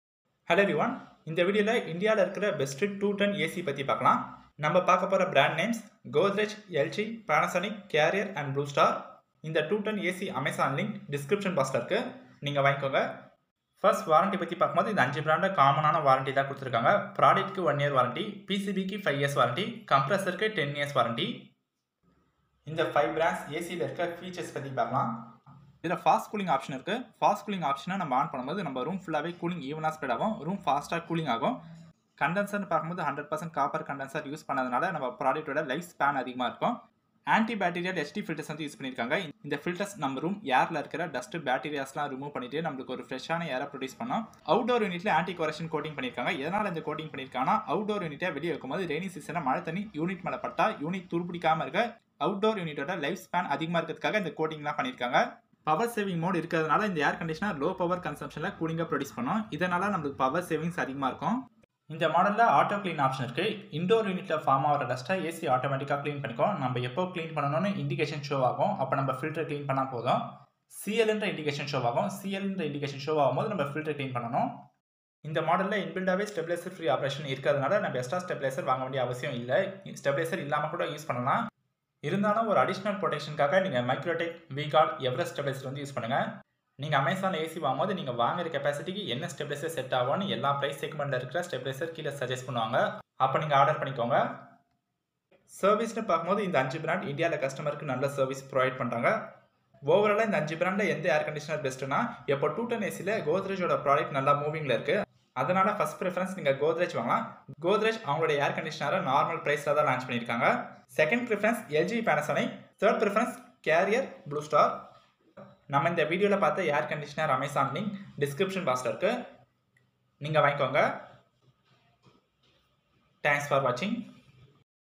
Best AC 2 Ton in India 2024 Model Tamil | Inverter Split Air Conditioner| LG Carrier Godrej bluestar
Godrej AC 🥶 👇
2 Ton 3 Star
2 Ton 5 Star
2.5 Ton 3 Star

LG AC 🥶 👇
2 Ton 3 Star
2 Ton 3 Star

Panasonic AC 🥶 👇
2 Ton 3 Star
2 Ton 4 Star

Carrier AC 🥶 👇
2 Ton 3 Star
2 Ton 3 Star
2 Ton 5 Star

Blue Star AC 🥶 👇
2 Ton 3 Star
2 Ton 5 Star

00:00 best 2 ton ac
00:14 warranty
00:24 features
01:36 service
01:39 which is best ac for home hall use

Hi friends in this video contains a best 2 ton ac in India Tamil 2024 model for big bed room home hall. Godrej 5-In-1 Convertible Cooling Inverter Split AC (Copper condenser I-Sense Technology 2024 Model AC 2T EI 24IINV3R32 WWR White)

LG  DUAL Inverter Split AC (Copper AI Convertible 6-in-1 Cooling 4 Way Swing HD Filter with Anti-Virus Protection 2024 Model TS-Q24ENXE White)
Panasonic Wi-Fi Inverter Smart Split AC (Copper Condenser 7 in 1 Convertible with True AI Mode 4 Way Swing PM 0.1 Air Purification Filter CS/CU-NU24ZKY4W White)

1 year on product comprehensive
5 year on pcb board
10 years on compressor

Carrier AI Flexicool Inverter Split AC (Copper Convertible 4-in-1 Cooling Dual Filtration Auto Cleanser ESTER Exi - CAI24ES3R32F0 White
Blue Star Convertible 5 in 1 Cooling Inverter Split AC (Copper Multi Sensors Dust Filter Smart Ready Blue Fins Self Diagnosis 2024 Model IC524DNUR White)

room size: 150 to 250 sq ft.